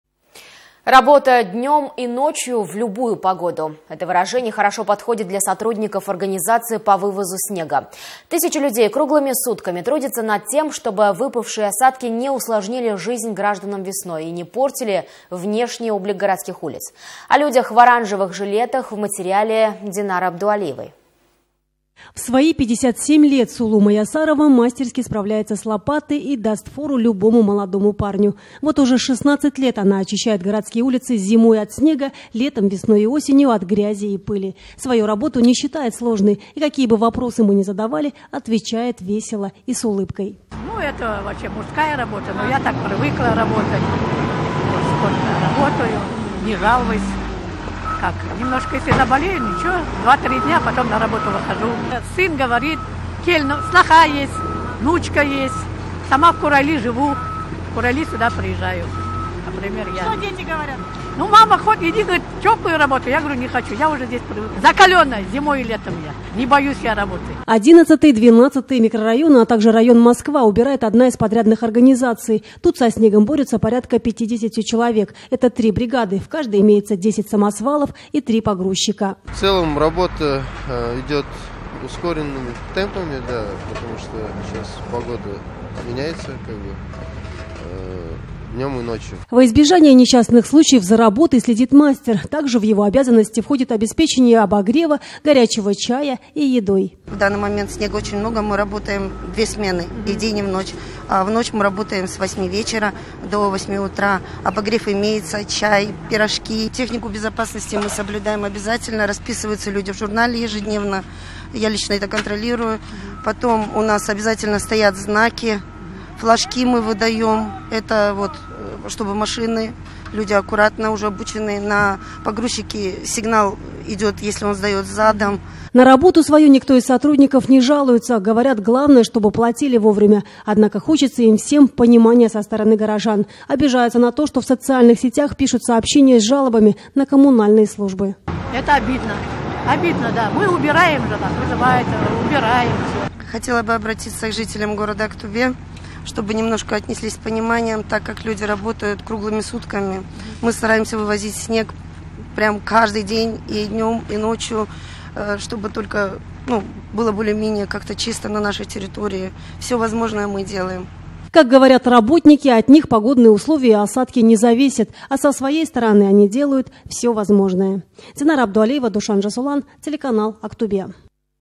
Неженский труд
Работа днем и ночью в любую погоду. Это выражение хорошо подходит для сотрудников организаций по вывозу снега. Тысячи людей круглыми сутками трудятся над тем, чтобы выпавшие осадки не усложнили жизнь гражданам весной, и не портили внешний облик городских улиц.